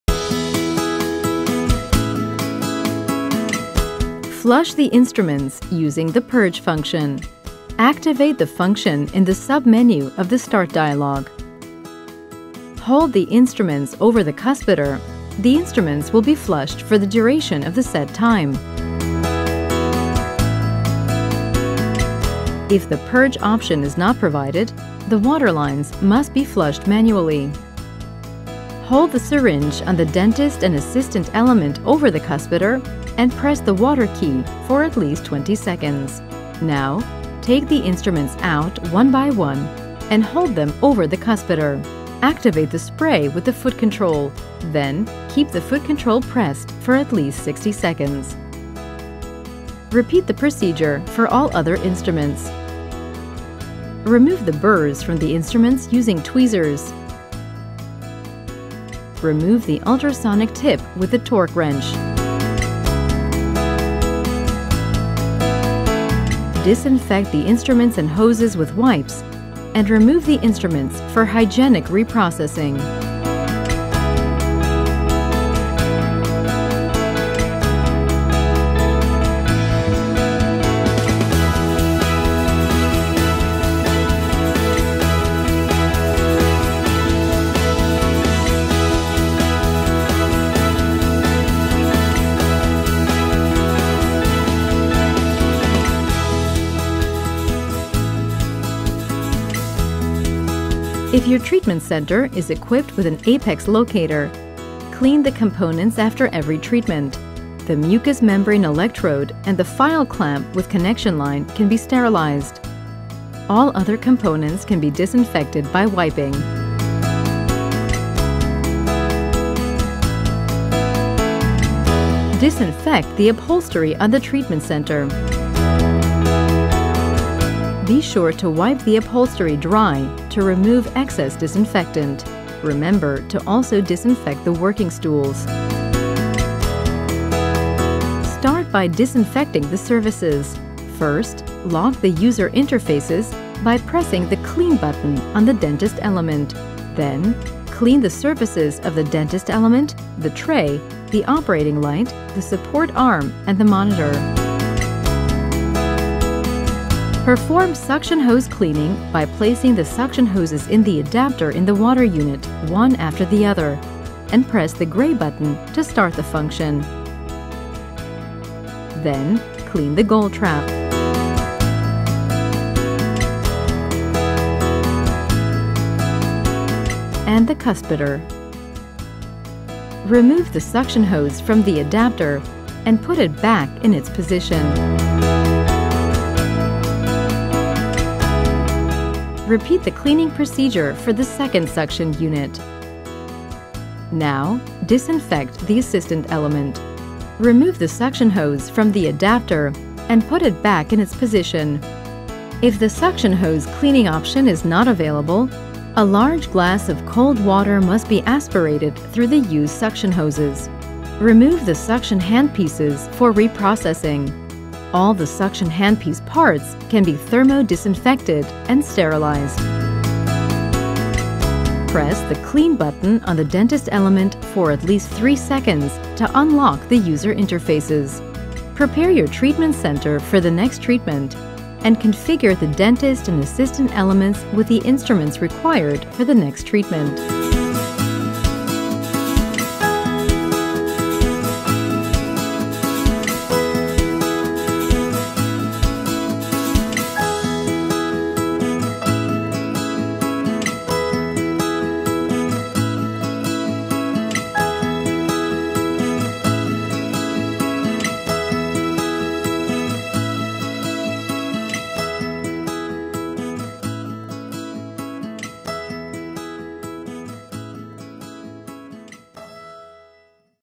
Infection Prevention for Treatment Centers: Intego - Post-Patient Protocol
Discover best practices for infection prevention in treatment centers with the Intego treatment unit. Learn essential steps and procedures to follow after each patient visit to maintain a clean and safe environment. Join us as we delve into the comprehensive protocol for disinfection and sterilization, ensuring optimal hygiene standards in your dental practice.